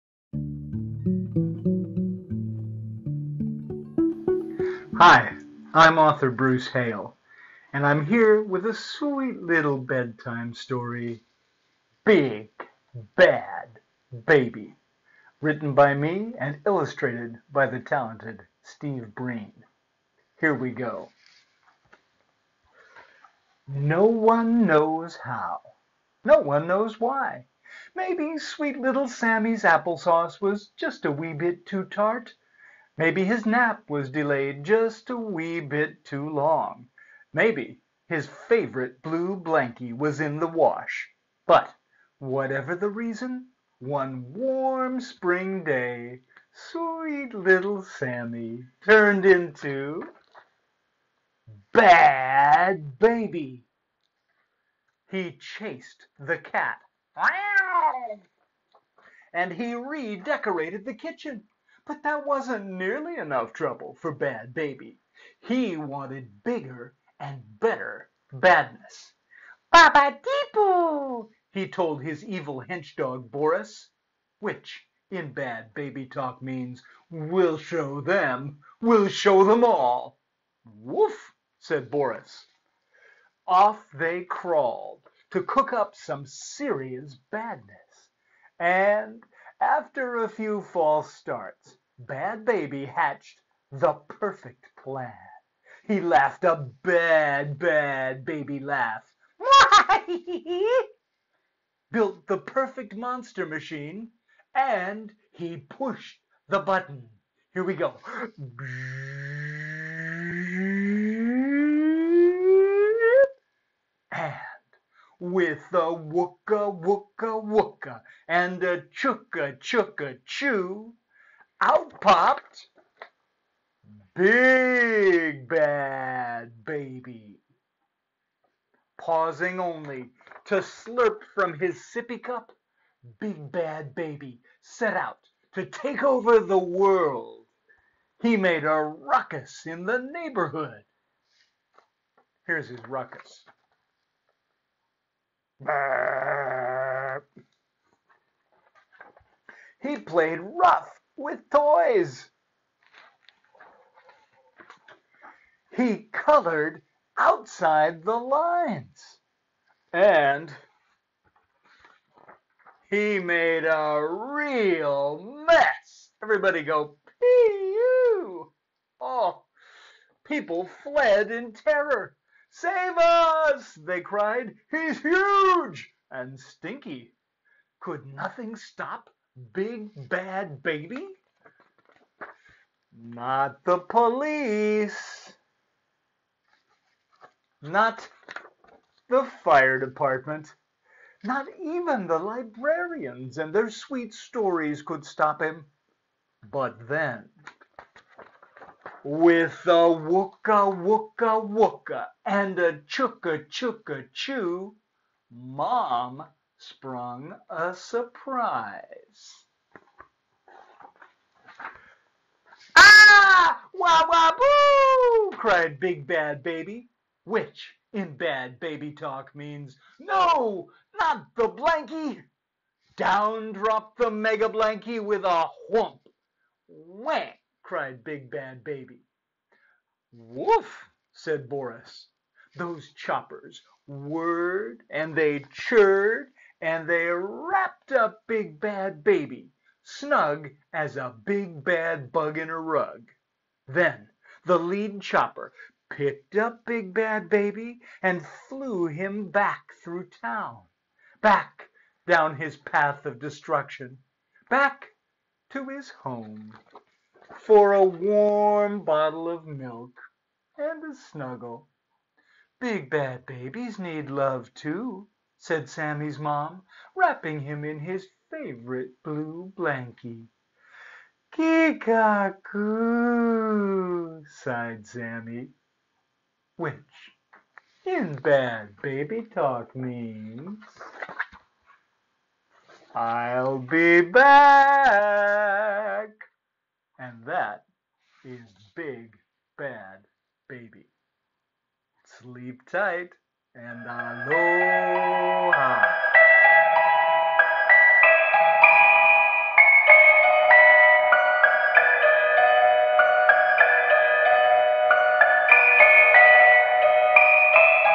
Storytime: Big Bad Baby
Author Bruce Hale reads his book, Big Bad Baby, illustrated by Steve Breen, and published by Dial Books for Young Readers.